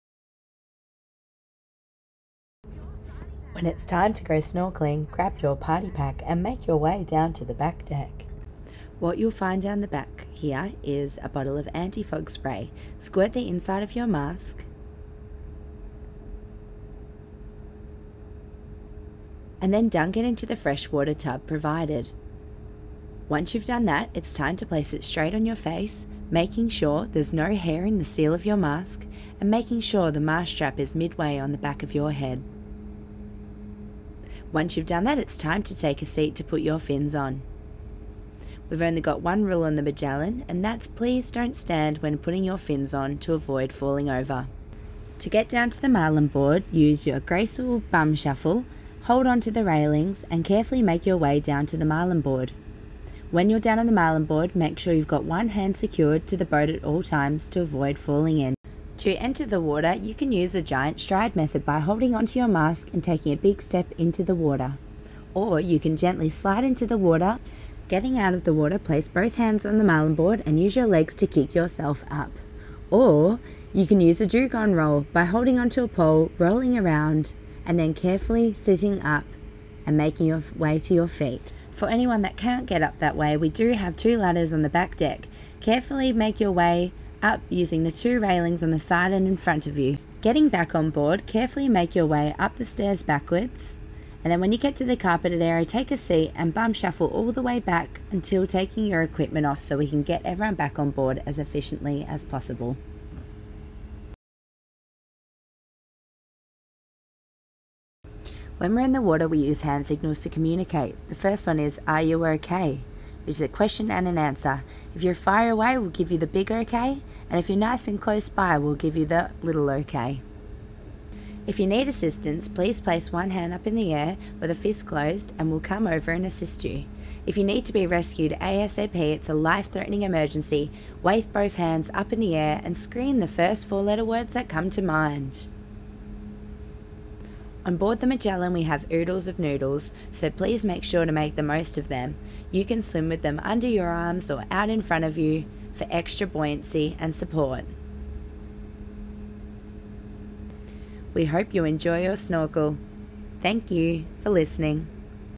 Kings Ningaloo Reef Tours Snorkel Brief
Our tours involve snorkelling the amazing Ningaloo Reef. To assist our customers to get the best out of their snorkel experience we have this short clip showing you how to correctly put on your equipment but also how to get in and out of the water safely. Our professional guides will always be on hand to offer assistance. But hopefully this clip will provide a few handy tips that you can use on the day.